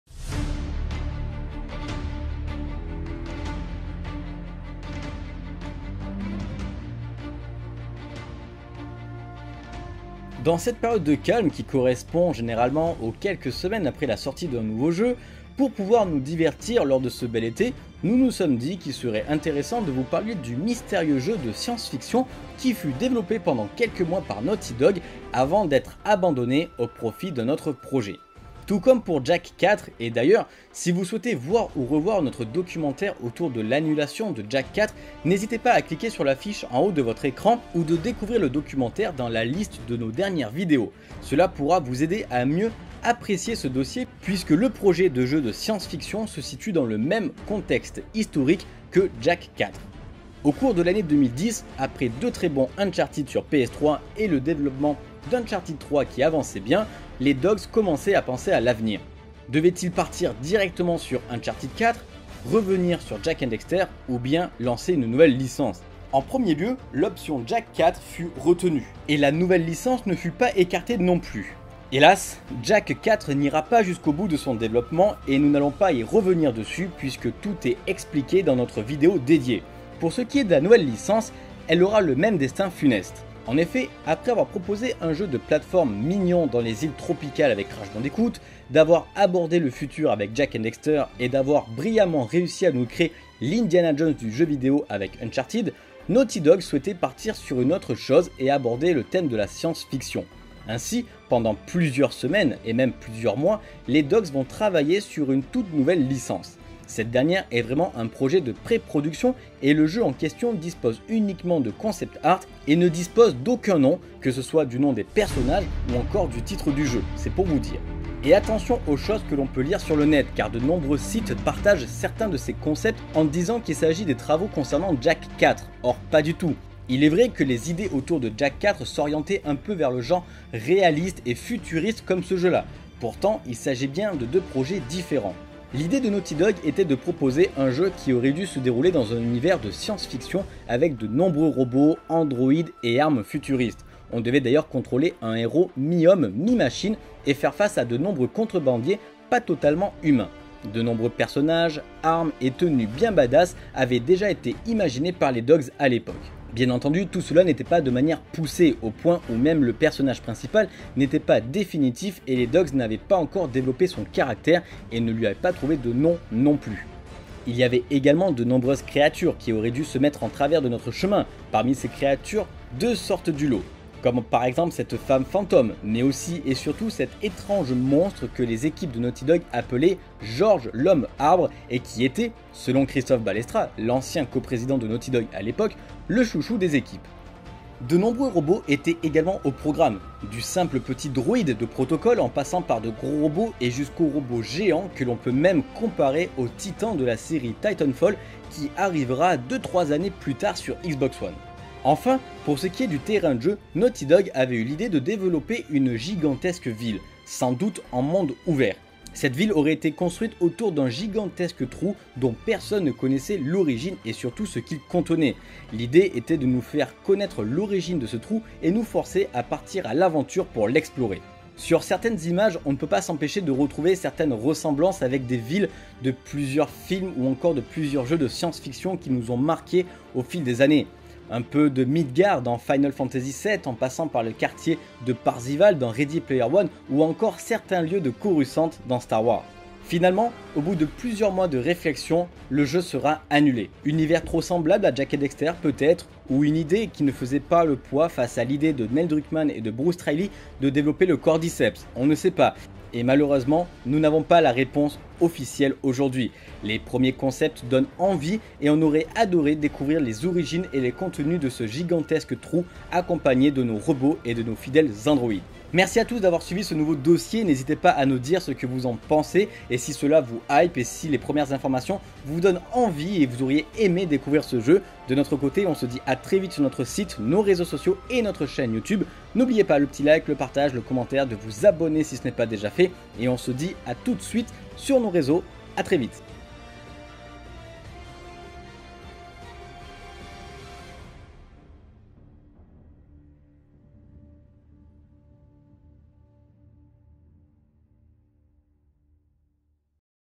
LE JEU DE SCIENCE FICTION ANNULÉ DE NAUGHTY DOG (2010-2012)
En plein développement d'Uncharted 3 : L'Illusion de Drake (2010) et juste avant le développement de The Last Of Us (2011), Naughty Dog travaillait sur deux projets à savoir Jak 4 et un mystérieux jeu de science-fiction, à destination de la PlayStation 3. Hélas, les deux projets ont été annulé par les Dogs et nous pouvons découvrir seulement quelques concepts art qui font drôlement envie !

CHRIS - LIBERTY93 :
_ Facebook :

ROCKSTAR MAG' :
_ Facebook :